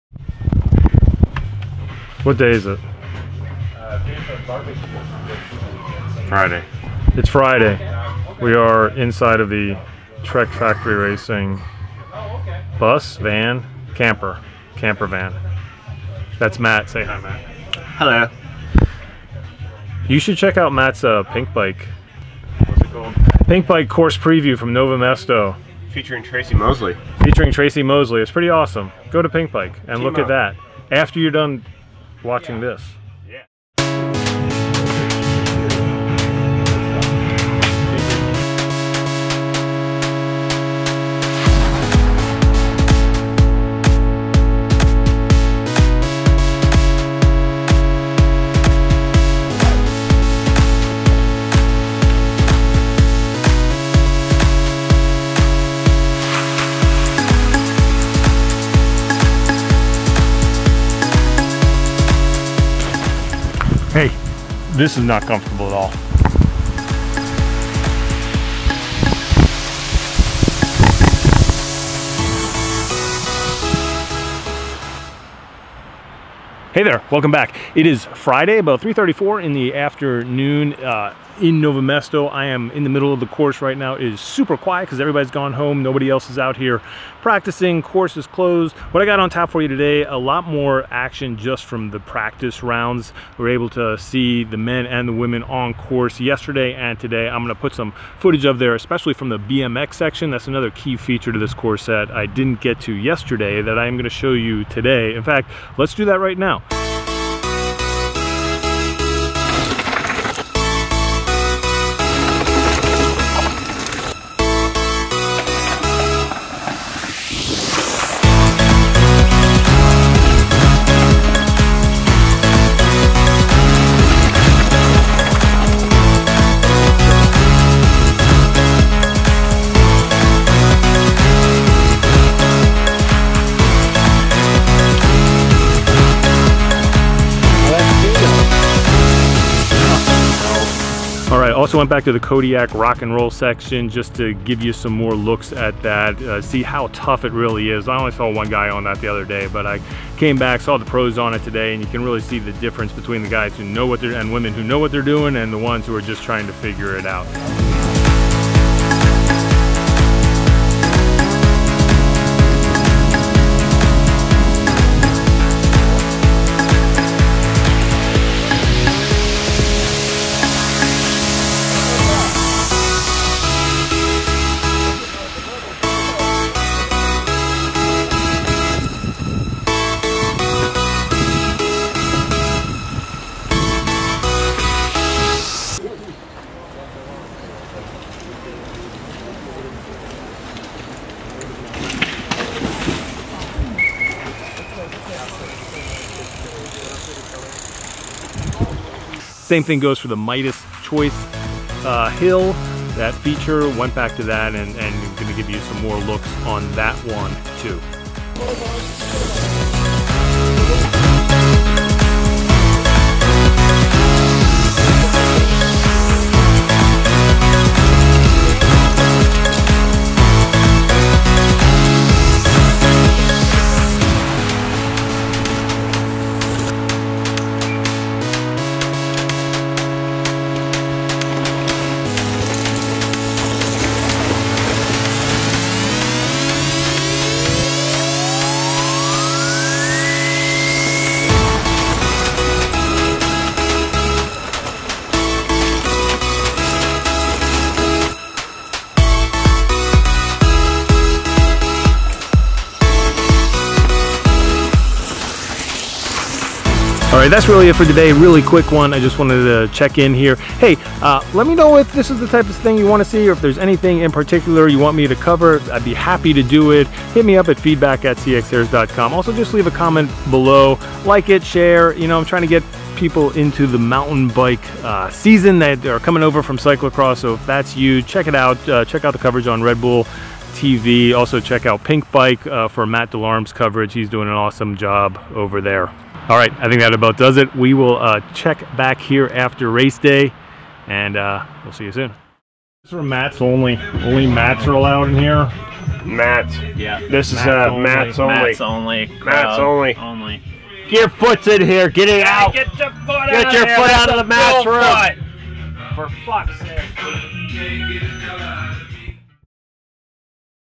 FFR 1.4 | More From Nové Mesto
We take a look at more course features in front of the weekend's World Cup XCO races.